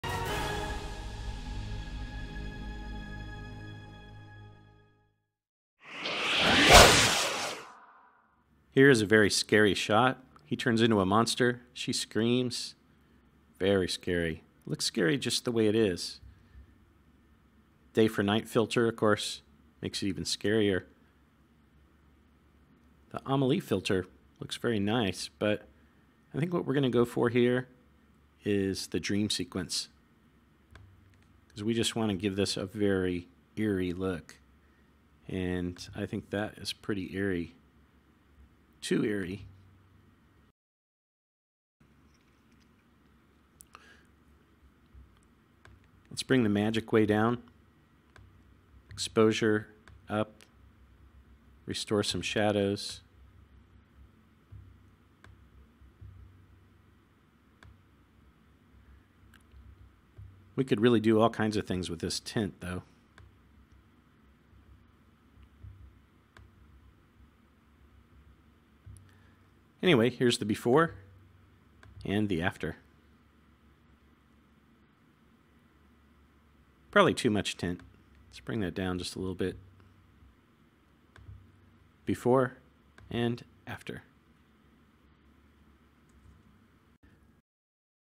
FilmStyles scary "Dream Sequence" filter demo in FCP X
Watch how we use the FilmStyles "Dream Sequence" filter to create an eerie, dream-like effect with tints and glowing highlights.

Get the FilmStyles filter bundle for Final Cut Pro X